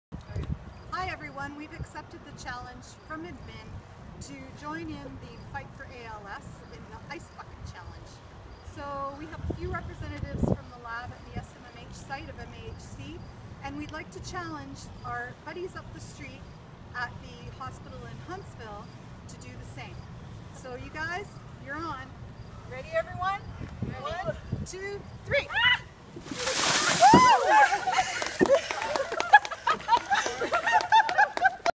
SMMH Lab ALS Ice Bucket Challenge
Laboratory Services staff at the SMMH Site accept the ALS Ice Bucket Challenge from the Senior Leadership Team and Human Resources Department at MAHC. In return, they challenged the Laboratory Services staff at the HDMH Site.